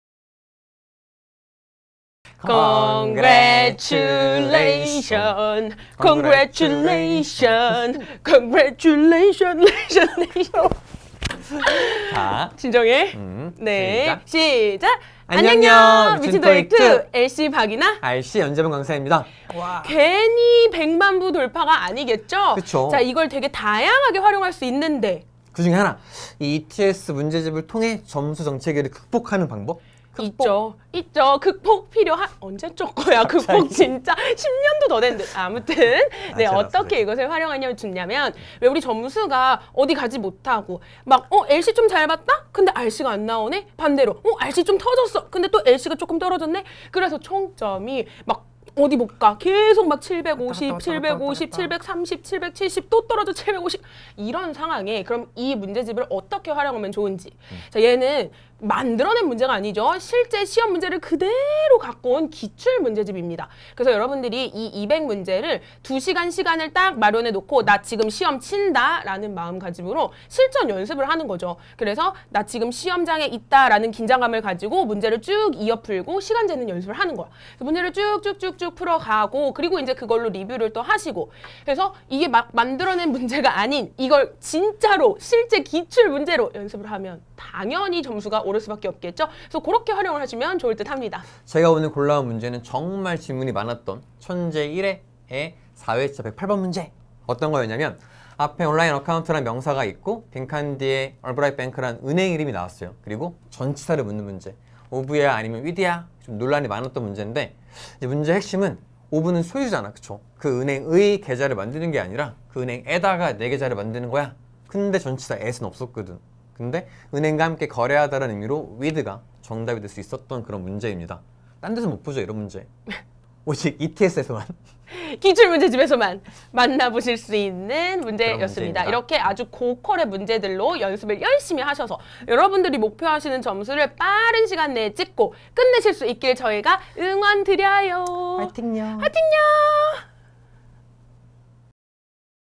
[토익기출 100만부 돌파] 고사장 긴장감 만드는 방법📜 | 논란의 기출문제 핵심풀이법 | YBM인천센터 미친토익2
★ ETS 토익기출 100만부 돌파 기념 ★
내가 뽑은 토익기출 원픽 - YBM인천센터 미친토익2 편
점수 정체기에도 해결방법은 역시 ETS 기출!

미친토익2 쌤들이 골라온 틀리기 쉬운 기출 원픽문제, 영상에서 확인해보세요.

*문제출처: ETS 토익 정기시험 기출문제집 1000 RC Vol.1 - TEST 4. 108번

*100만부 대상: ETS 토익 정기시험 기출문제집 3종, 기출 종합서, 기출 입문서, 기출 VOCA
*100만부 기준: 2017.12 ~ 2020.04 누적판매